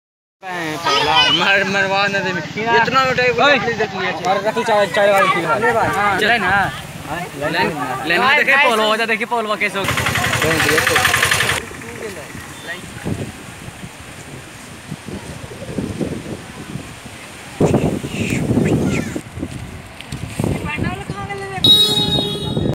incredible village mocharim
2016 August 12 ko aaye huye badh (niranjna  river )